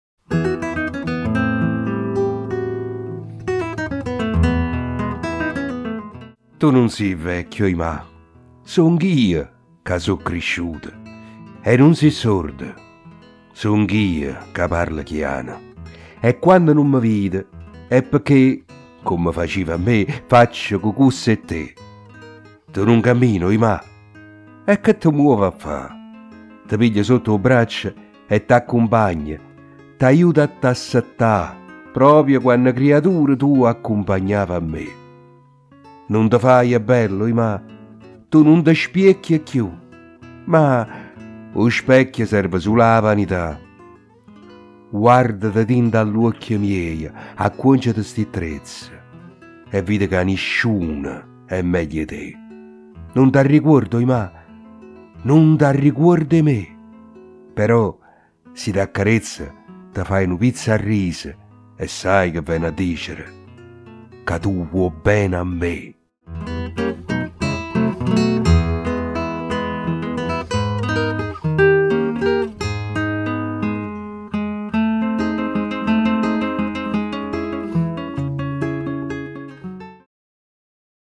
QUANNO TENEVO 'A MAMMA di Carlo Fedele - Le videopoesie di Gianni Caputo
Tu nun sì vvecchia, oj mà,
songh' i' ca so' crisciuto.
E nun sî sorda,
songh' i' ca parlo chiano...
E quanno nun me vide
è pecchè, comme facive a mme,
faccio cucù e settè.
Tu nun cammine, oj mà?
E che te muove a ffà!
Te piglio sotto 'o braccio e t'accumpagno,
t'ajuto a t'assettà,
proprio quanno criaturo
tu accumpagnave a mme.
Nun te faje bella, oj mà,
tu nun te spiecchie cchiù?
Ma 'o specchio serve sulo â vanità
Guardate dint' all'uocchie mieje,
accuoncete 'sti trezze
e vide ca nisciuno è meglio 'e te.
Nun t'arricuorde, oj mà,
nun t'arricuorde 'e me?
Però si t'accarezzo
te faje nu pizzo a rriso
e saje ca vvene a ddicere?
Ca tu vuò bbene a mme.